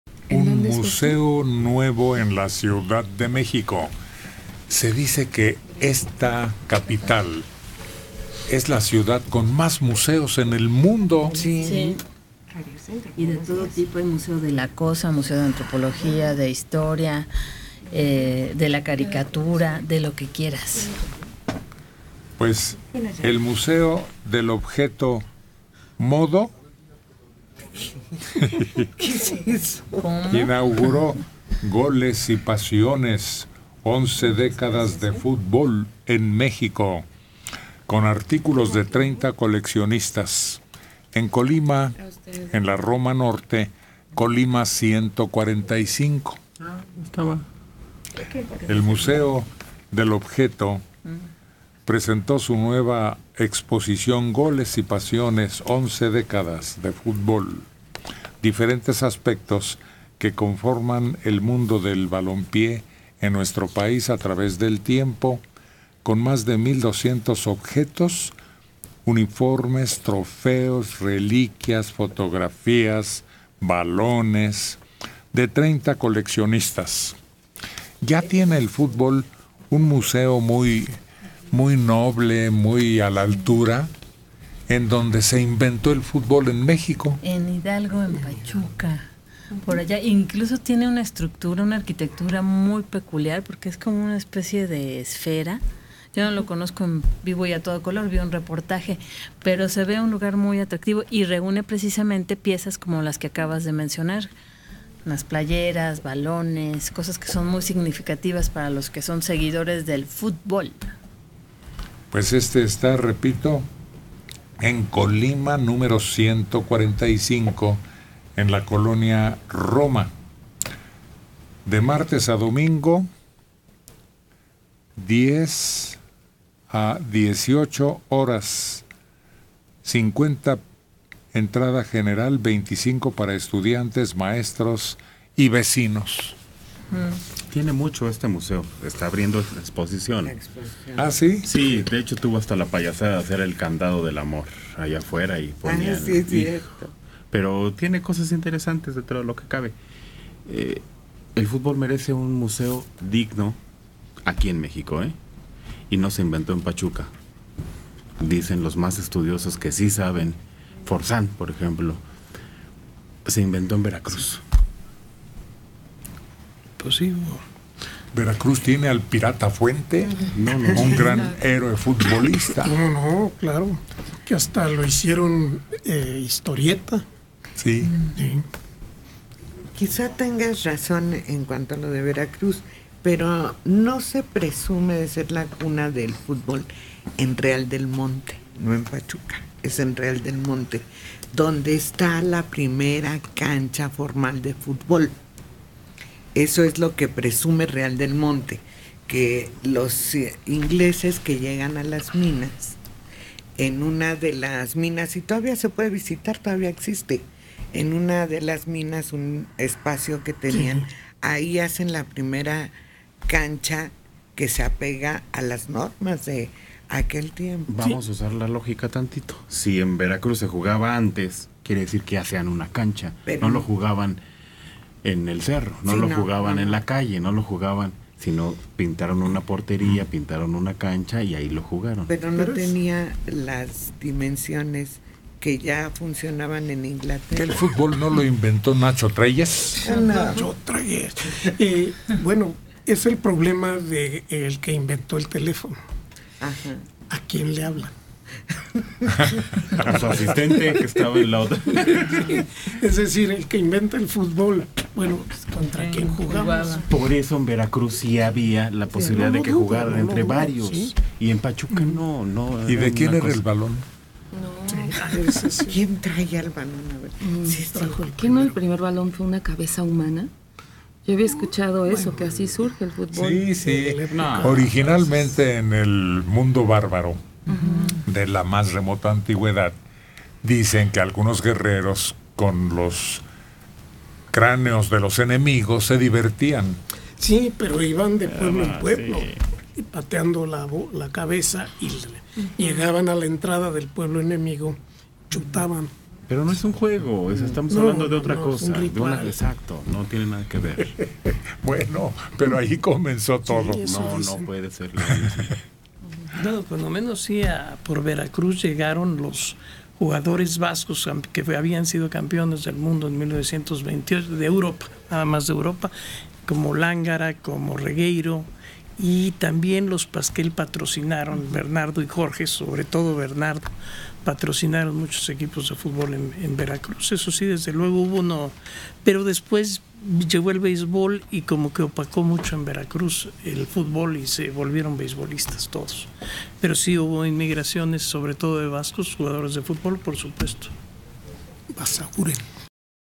En el Museo del Objeto: “Goles y Pasiones”, la exposición ideal para los amantes del fútbol
En el Museo del Objeto del Objeto: “Goles y Pasiones”, la exposición ideal para los amantes del fútbol
El Museo del Objeto del Objeto (MODO) es el primer museo en México dedicado al diseño y la comunicación. Fue inaugurado en el 2010, se basa en una colección de embalajes comerciales, dispositivos comunes y objetos recolectados por Bruno Newman por más de cuarenta años. El museo está dedicado a preservar más de 30,000 piezas provenientes de dos siglos, con la finalidad de la preservación de la colección y la promoción del coleccionismo en general. El museo forma parte de una organización sin fines lucrativos llamada igual que el con sede en una mansión de principios del siglo XX en la colonia Roma de la Ciudad de México.